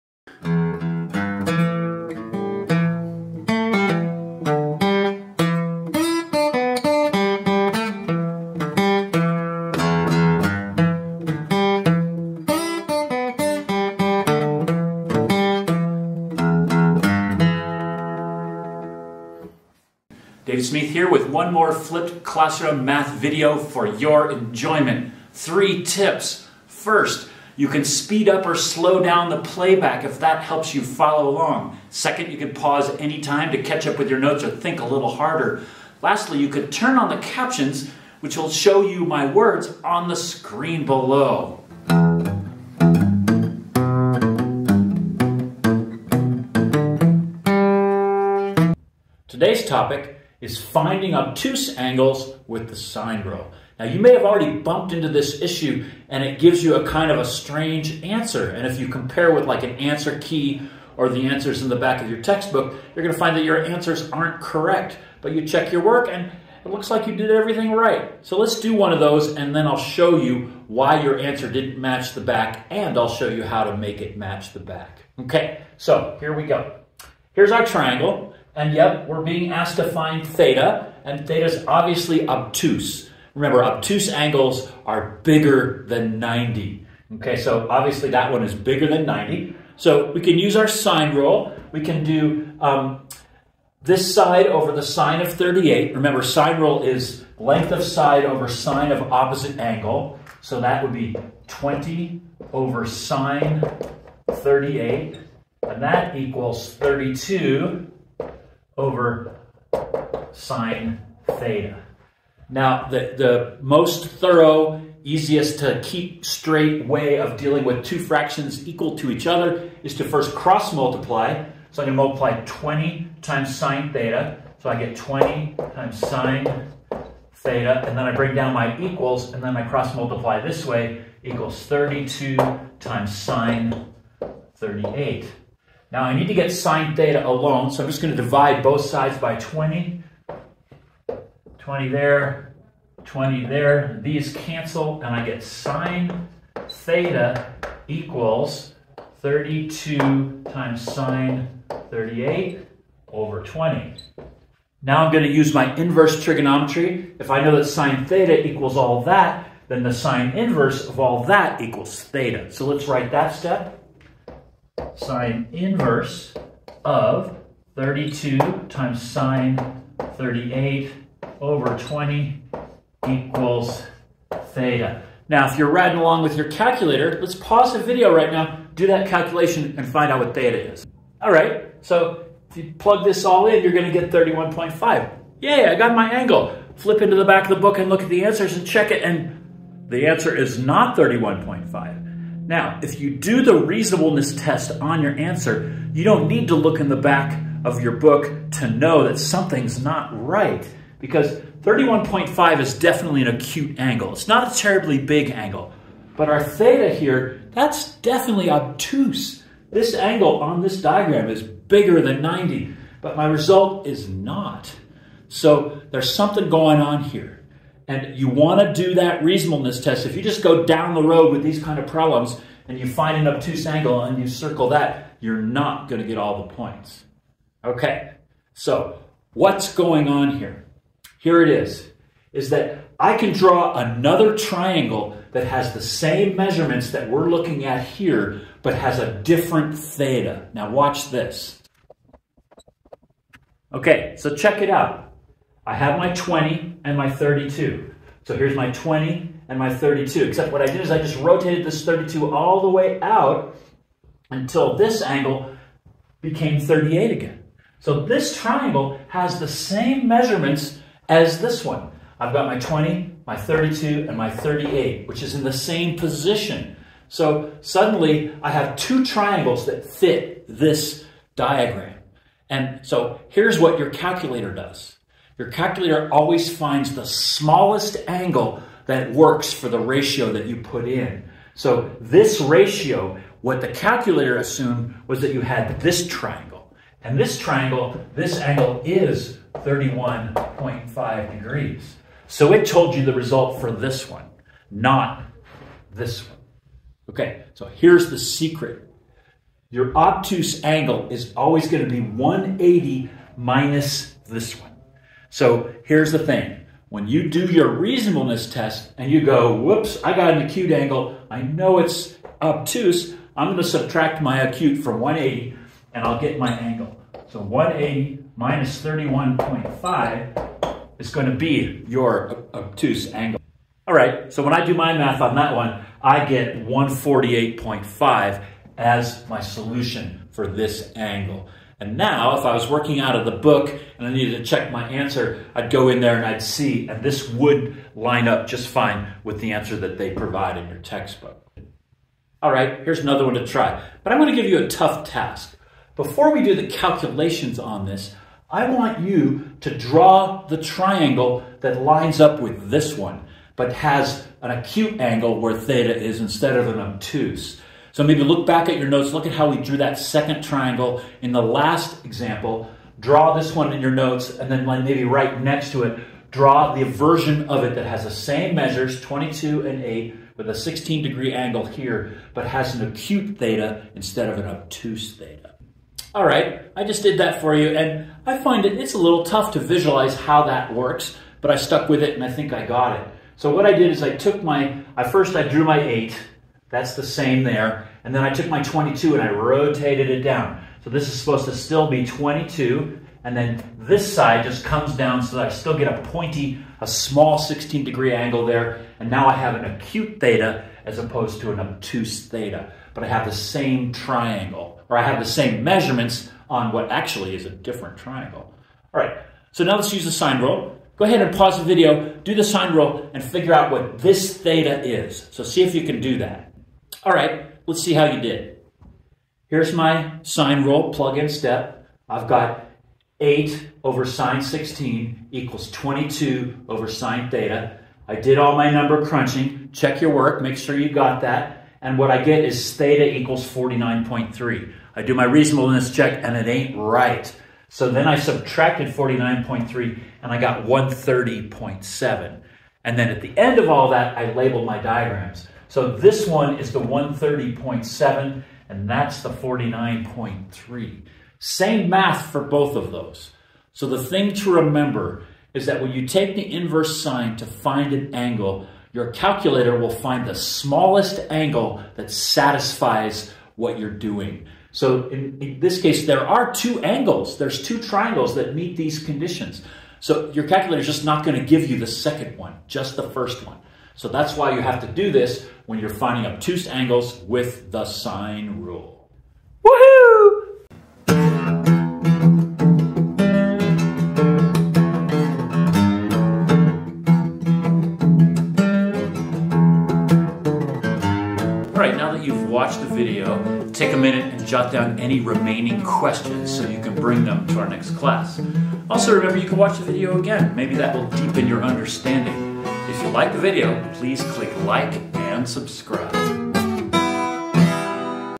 Using the Sine Rule to find Obtuse Angles
Finding obtuse angles with the sine rule can be tricky. This video explains how.